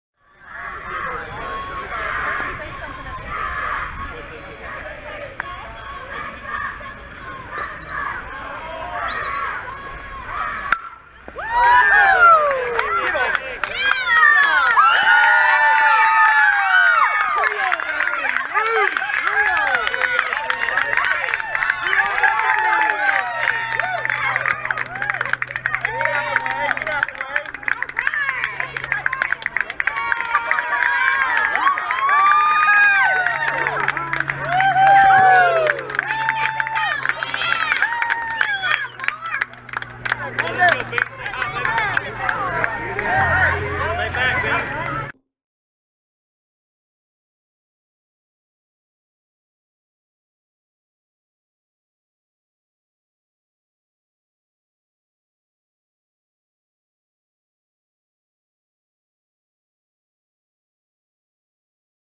04 30 2016 Landen Granger Homerun
Landen Granger hitting a homerun vs Oakleaf baseball in the USSSA 10u Florida-Georgia Showdown